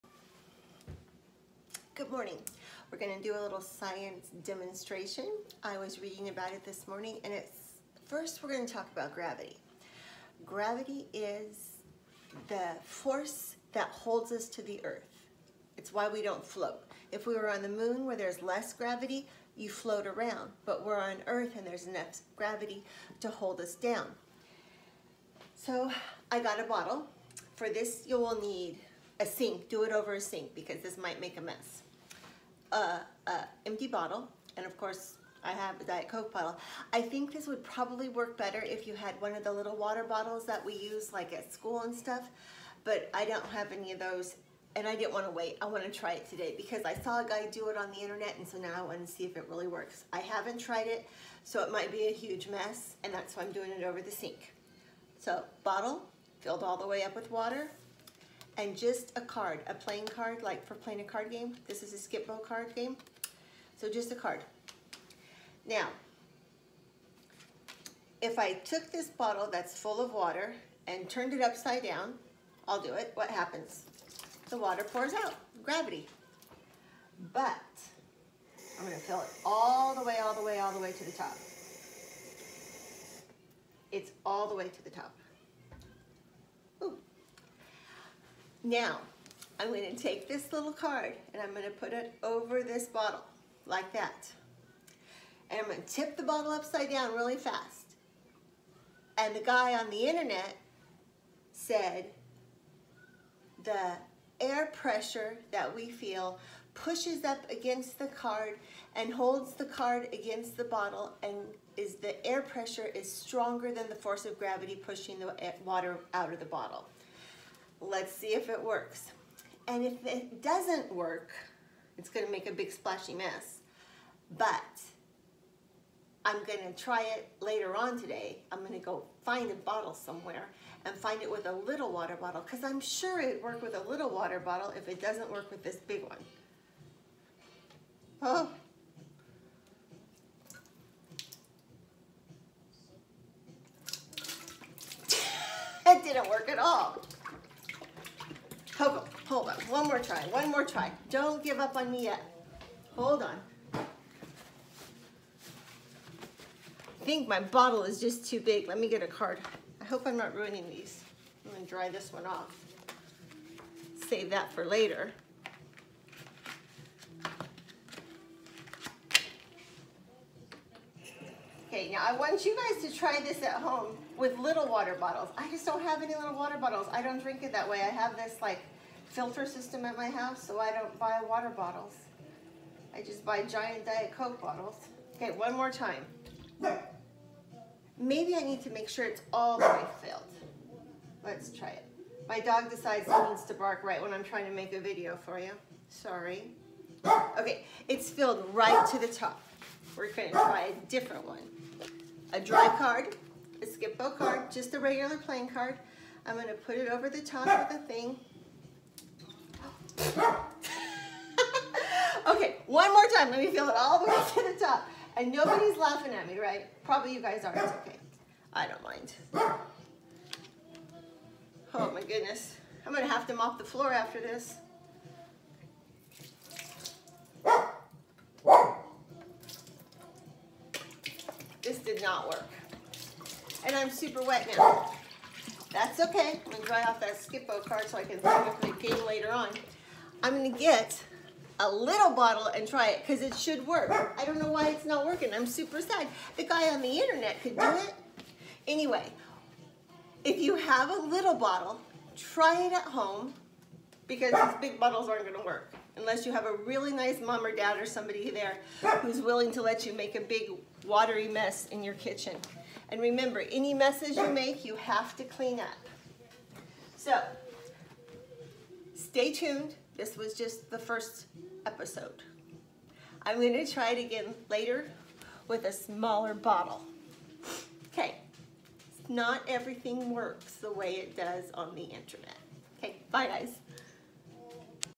Science demonstration FAIL
An attempt to recreate something I saw on the internet.